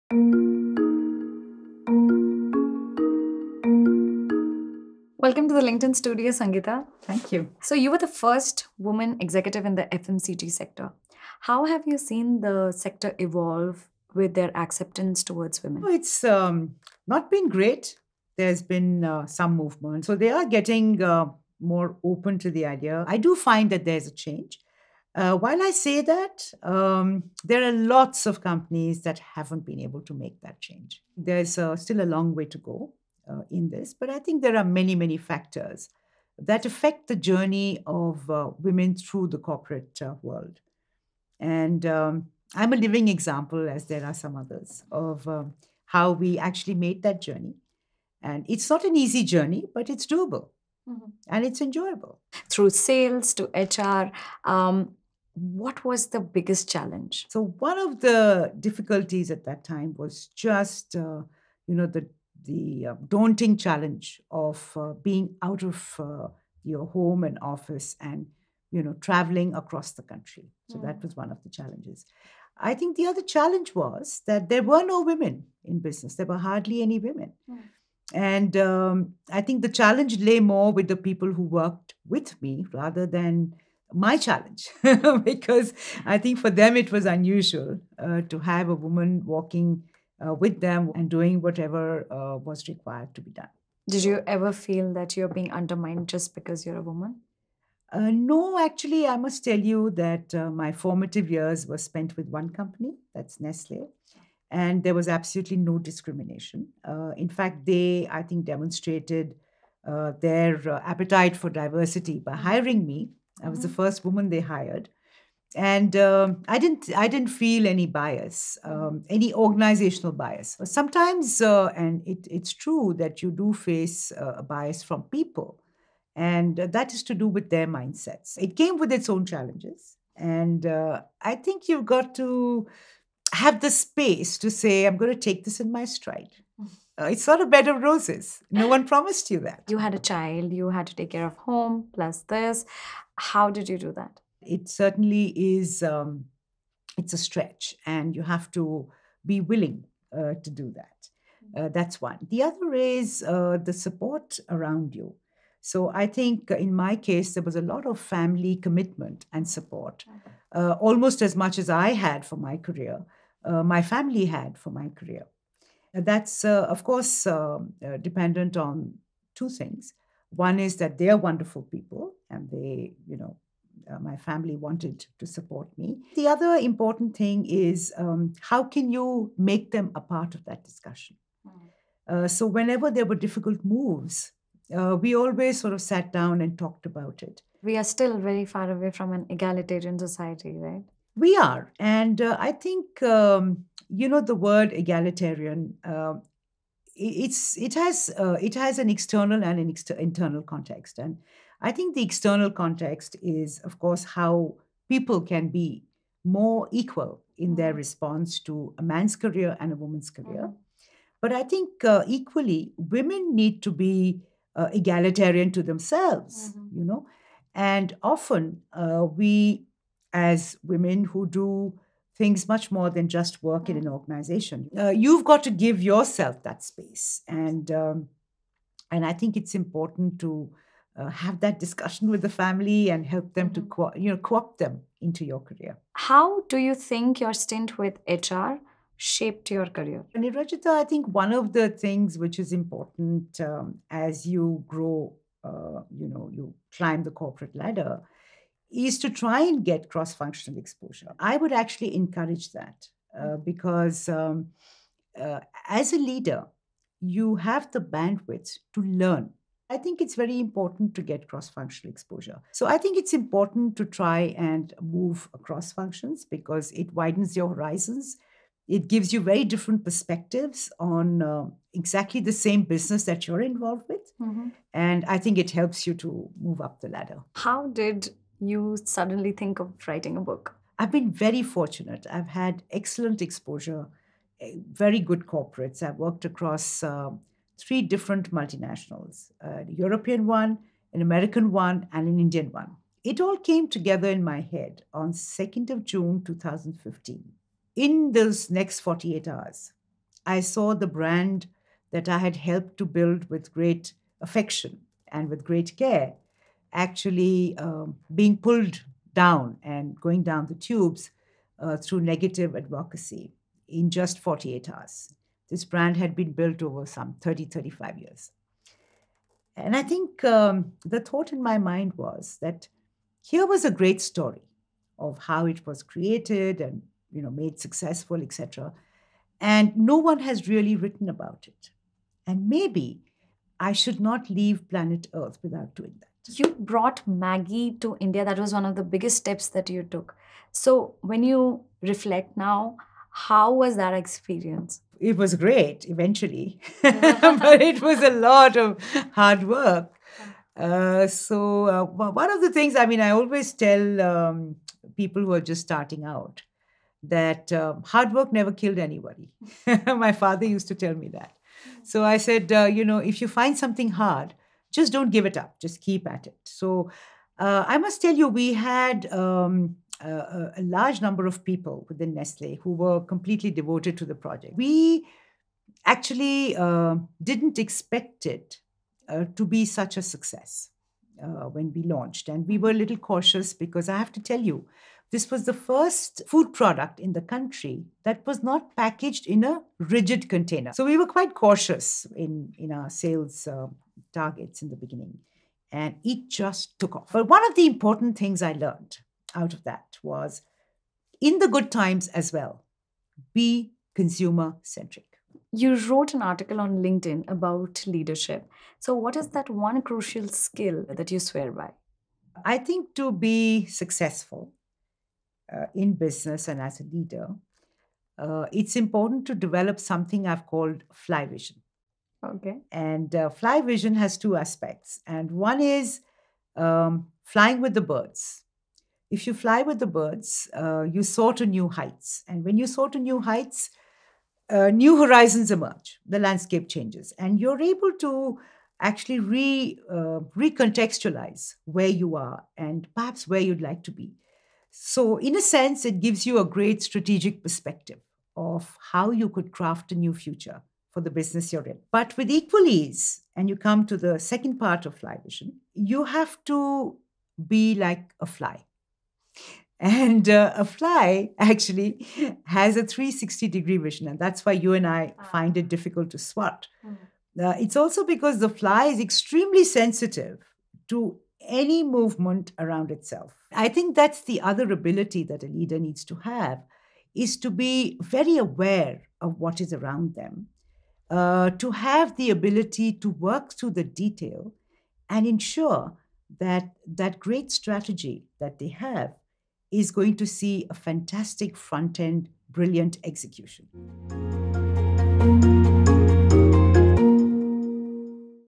In conversation with Sangeeta Talwar, Managing Partner, Flyvision Consulting LLP
Watch the conversation between our editor Nirajita Banerjee and Managing Partner at Flyvision Consultancy LLP, Independent Director and Author, Sangeeta Talwar, as she shares her journey in the industry so far and her views about the evolution of the FMCG industry. Sangeeta talks about the challenges faced during the starting years of her career and how a 2 minute revolution turned the tables.